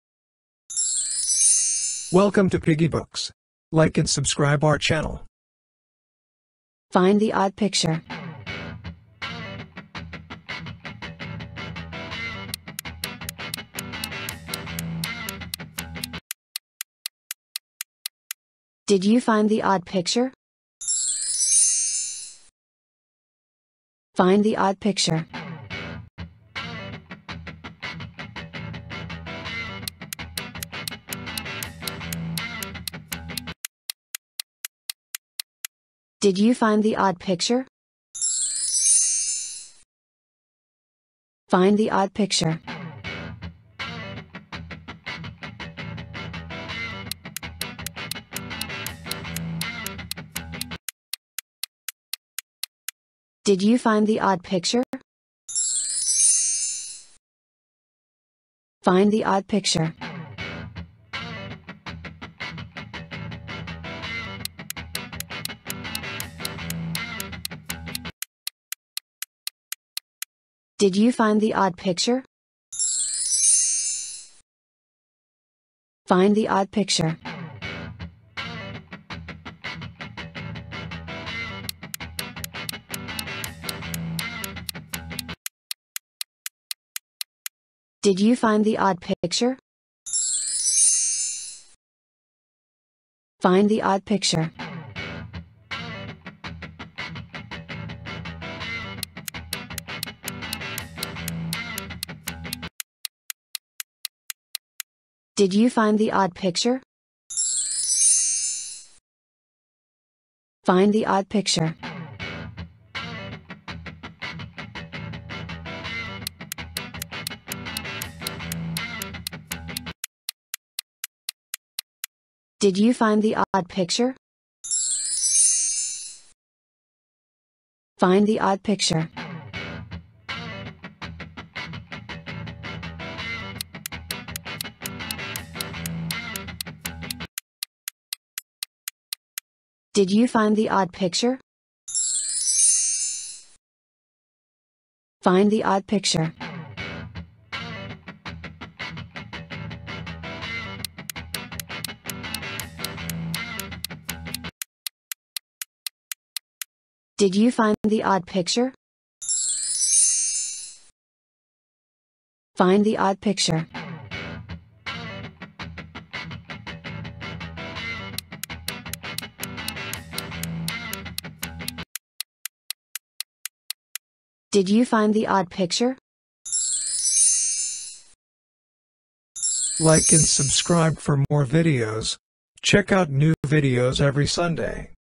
Odd one out for kids | Find the different pictures | Kids IQ test
Use this video to help your child understand the concept of odd one out or categories.